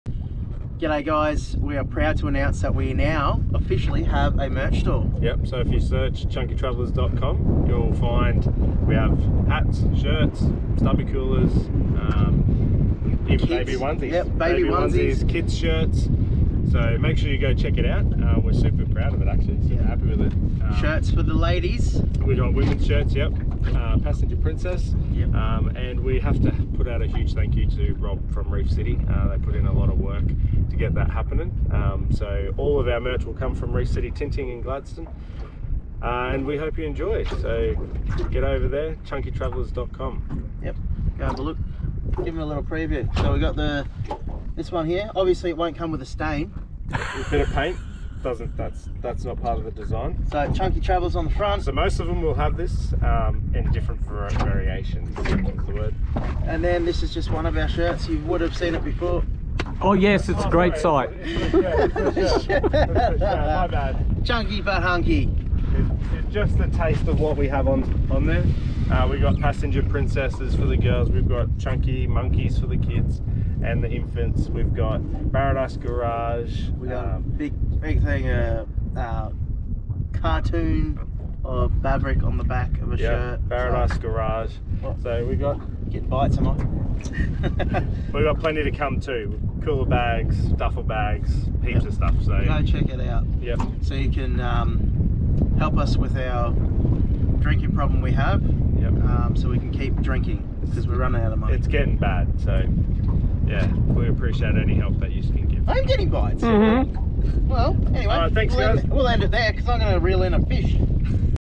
HUGE ANNOUNCEMENT We have a merch store!!! chunkytravellers.com
So pumped to share with you guys our merch store that is up and running, so go check out and get your hands on some of our first ever merch.